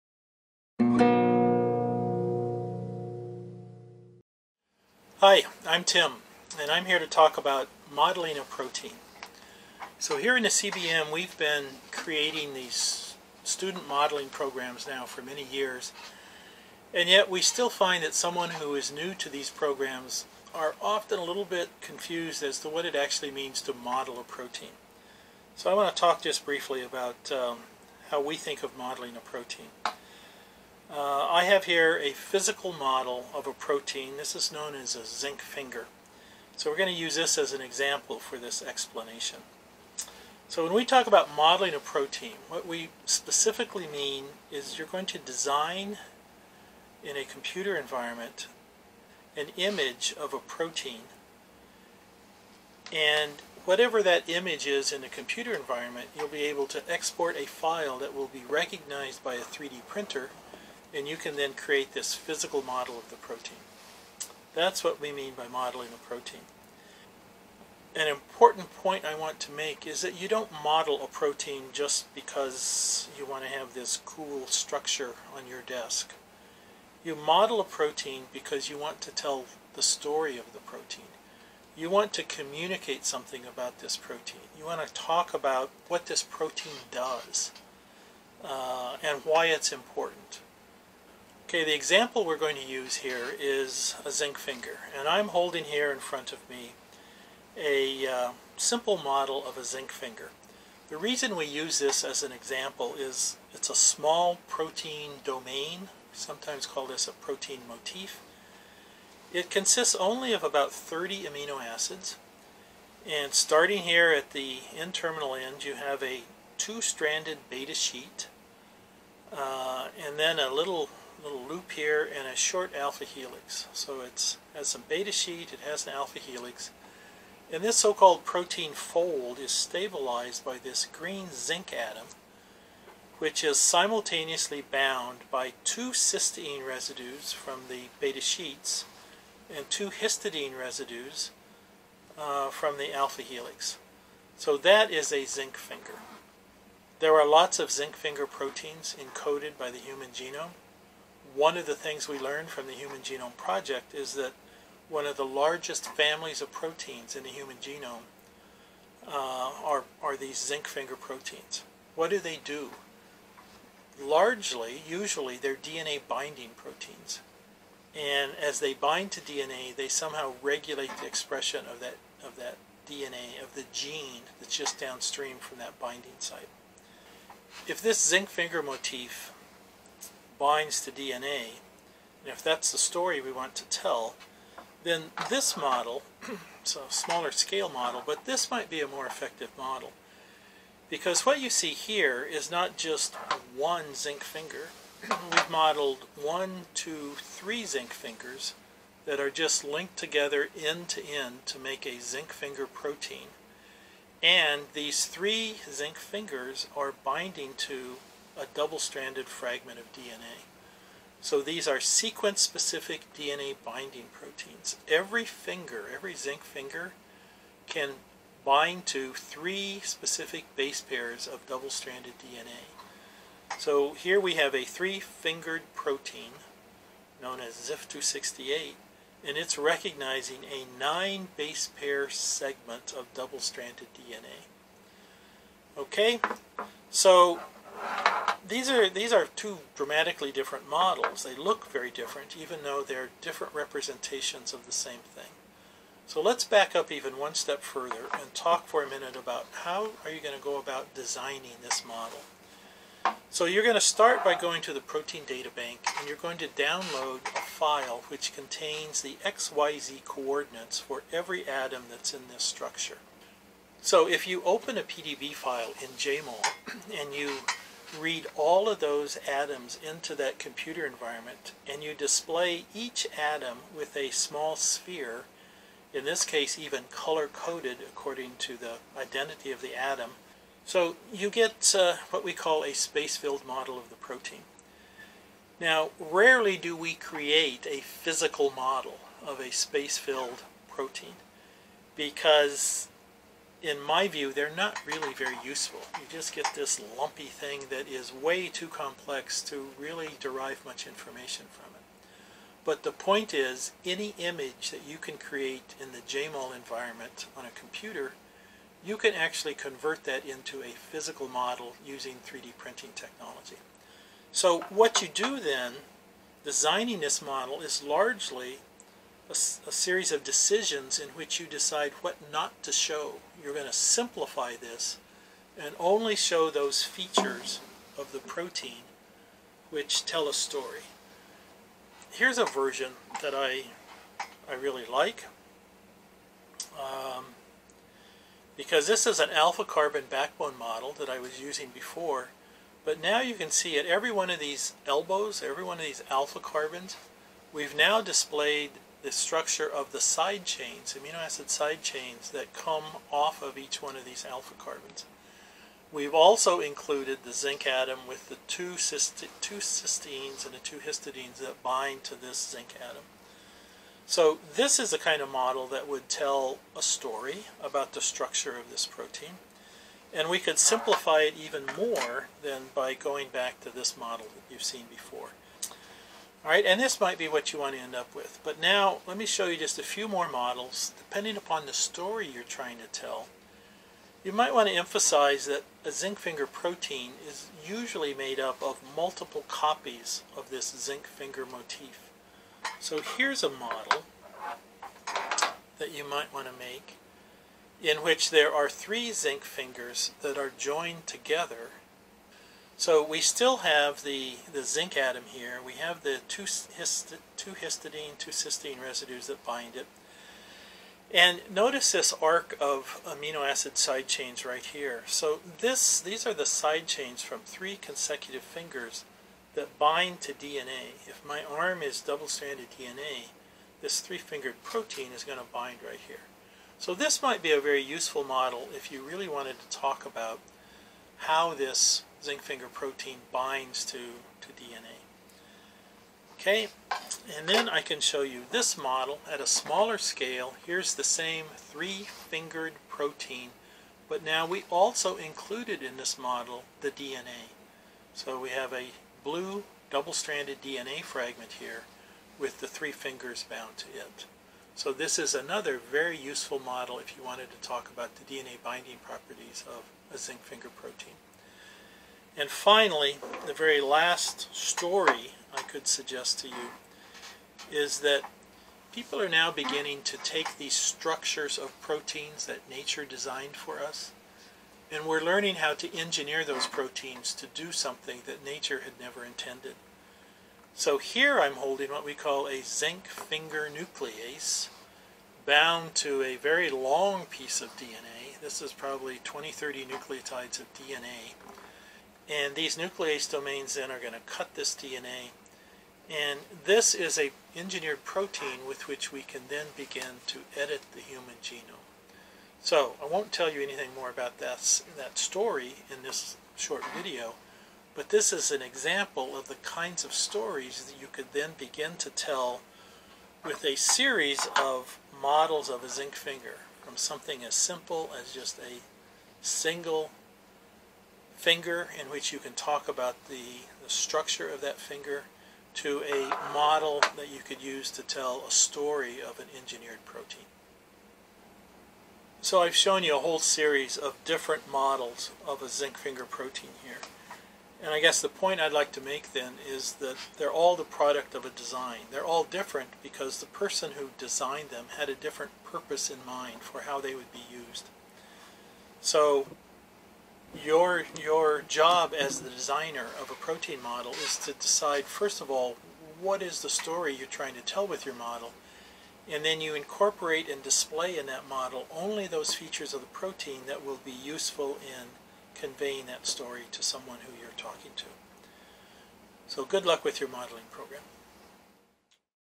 What Does it Mean to "Design" a Protein Model?
Tim Herman, PhD from the MSOE Center for BioMolecular Modeling describes what it means to "design" a protein model for 3D printing.